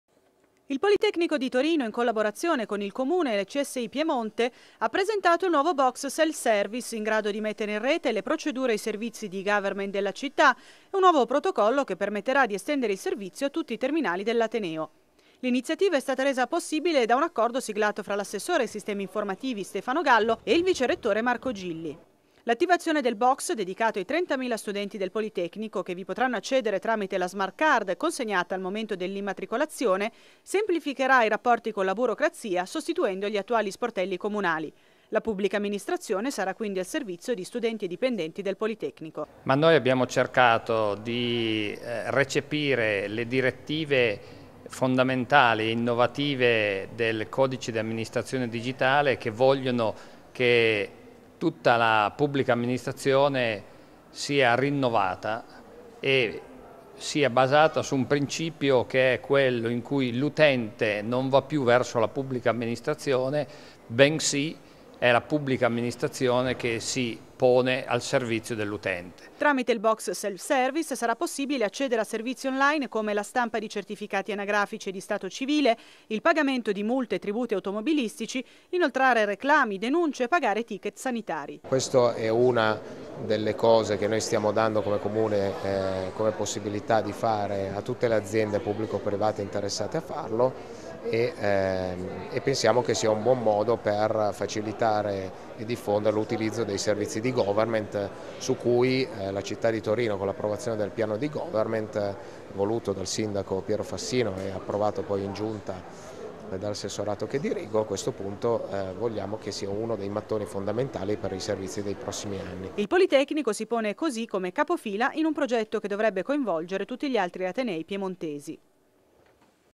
Box self service
Presentato al Politecnico di Torino il Box self service in grado di sostituire le funzionalità degli sportelli automatici.